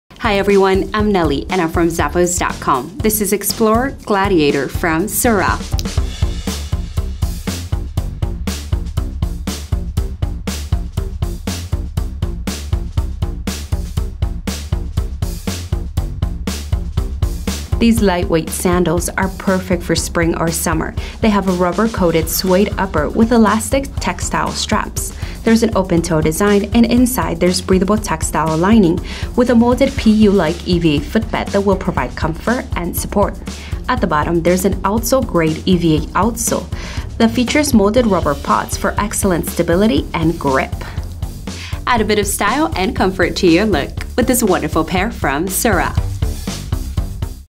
SOREL Explorer Gladiator SKU: 8975306
The SOREL&reg; Explorer Gladiator sandal offers a fierce style for your summer look!
Rubber-coated suede upper with elastic textile straps.
Removable, molded PU-like EVA footbed with soft textile topcover for comfortable underfoot support.
Outsole-grade EVA midsole with molded rubber insert.
Outsole-grade EVA outsole construction with molded rubber pods for excellent stability and grip.
Imported.